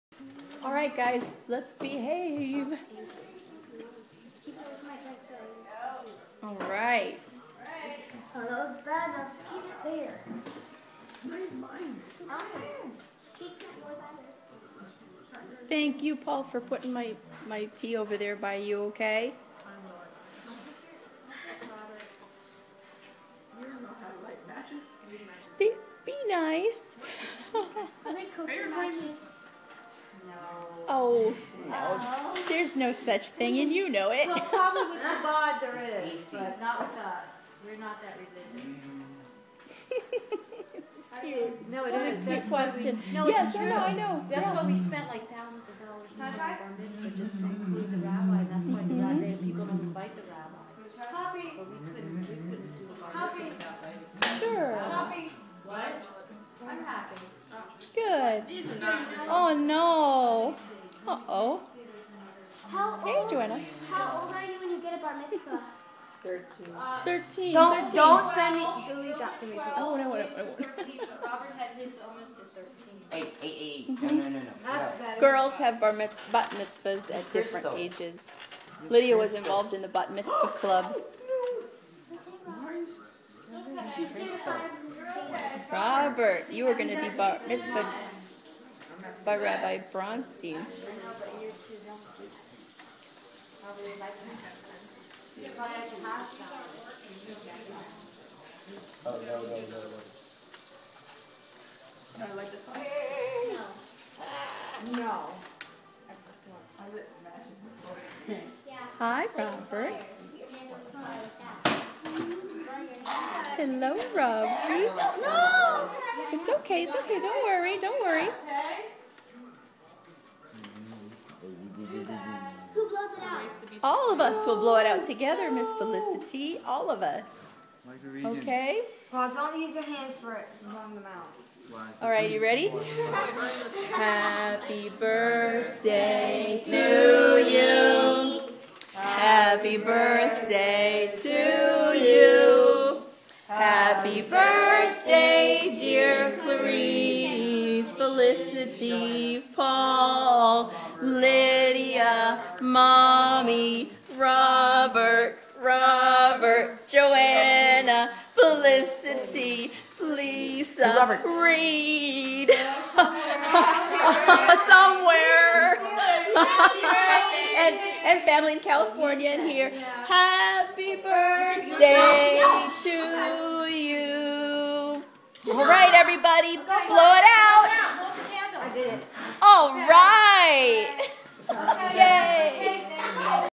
Happy Birthday Family
Celebrating a birthday party for all our dear relatives here and elsewhere.